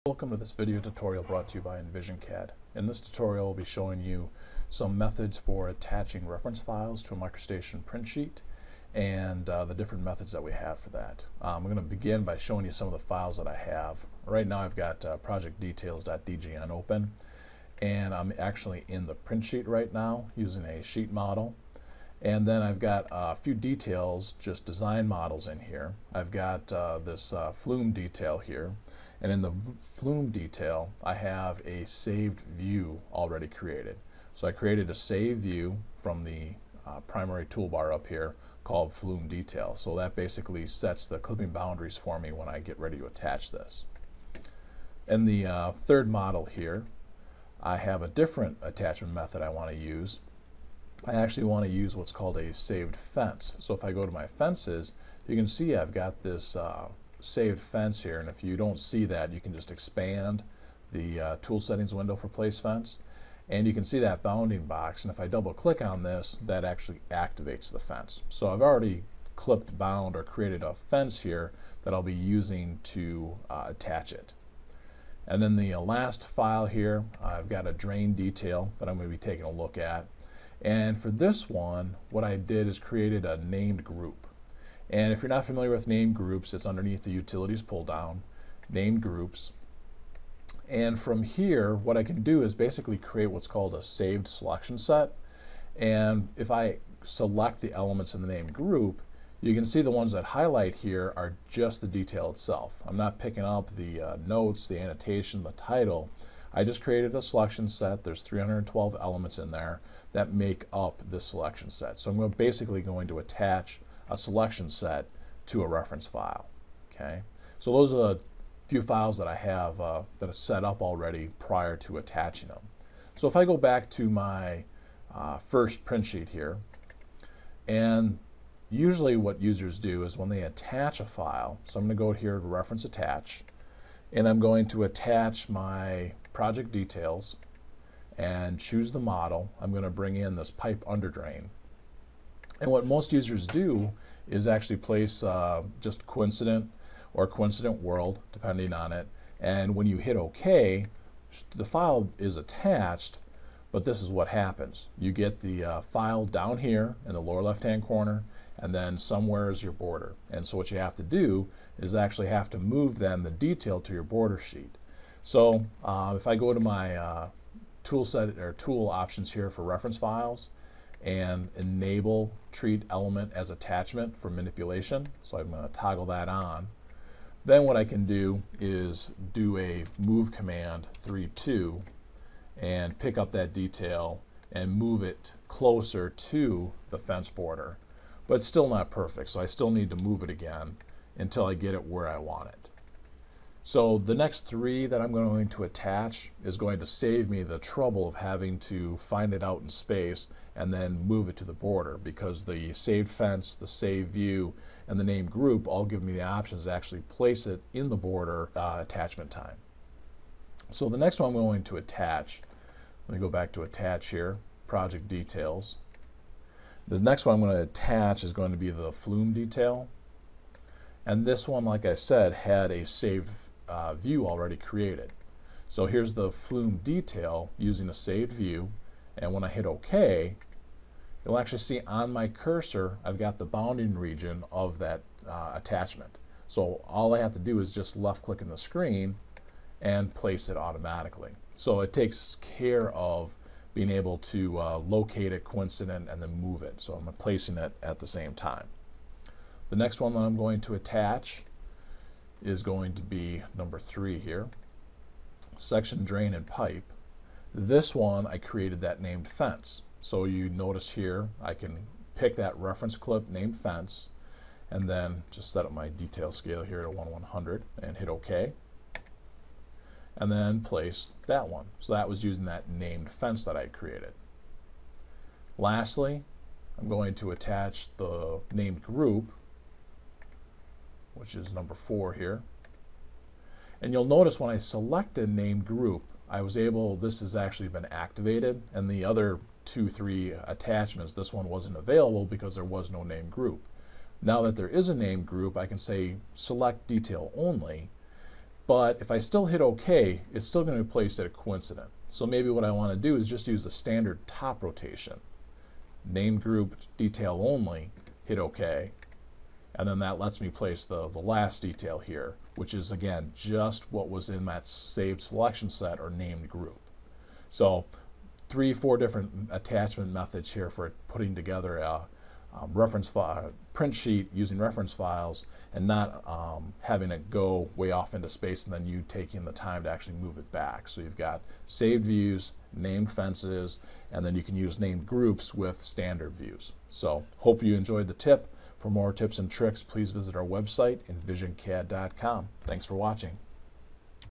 New Reference Attachment methods in MicroStation V8i (SELECTseries)
In this tutorial we will demonstrate how to attach a reference file using different methods.  First we will begin with Coincident and Coincident - World then move the attachment after placement using a new option that allows the reference to be manipulated as an element.  Then three more reference attachments will be demonstrated using Saved Views, Named Fences, and Named Groups using Standard View rotation.